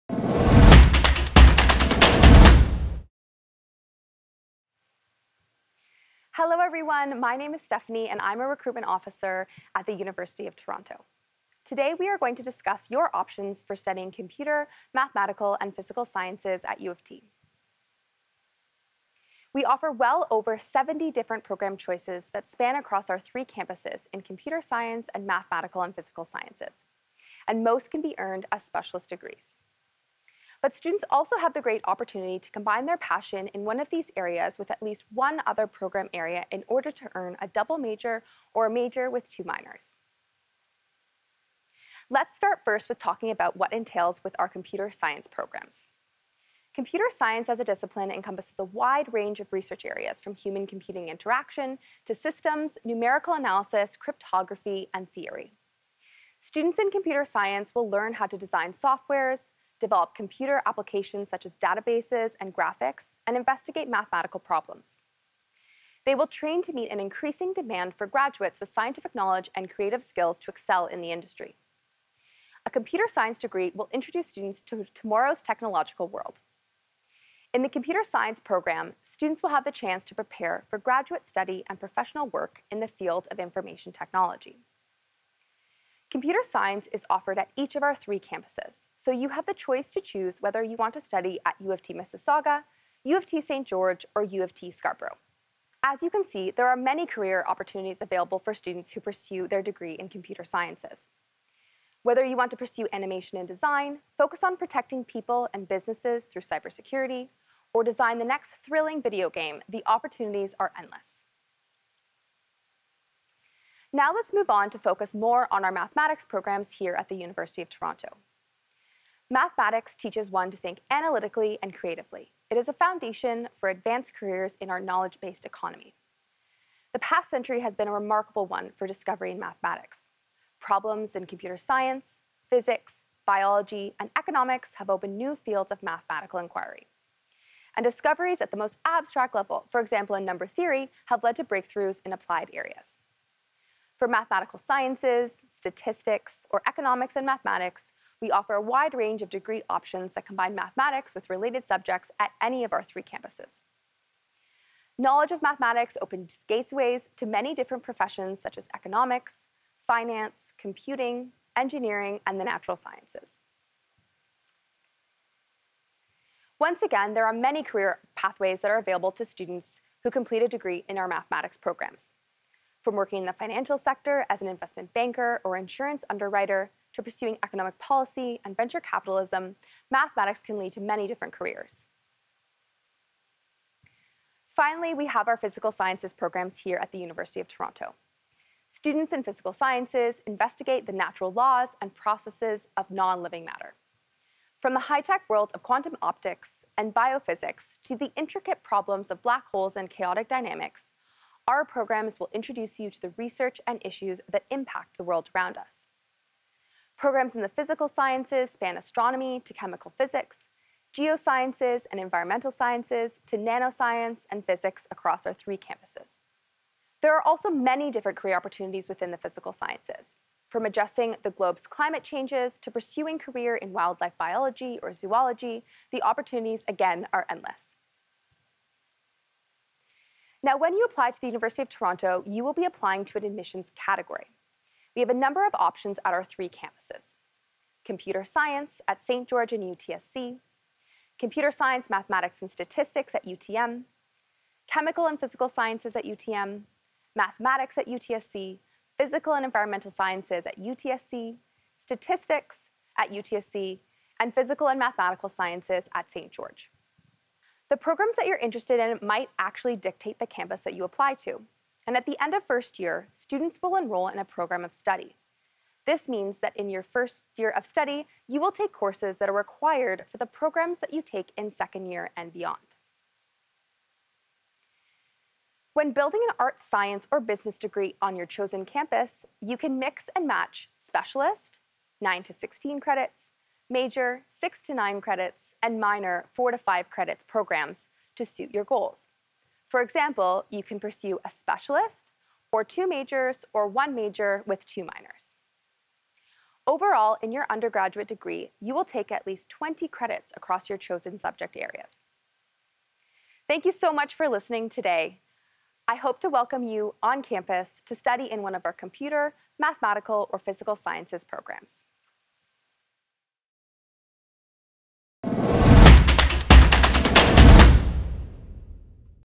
Learn About Undergraduate Programs in Computer, Mathematical, ​and Physical Sciences at U of T
Learn more about undergraduate computer, mathematical, and physical science program options at the University of Toronto.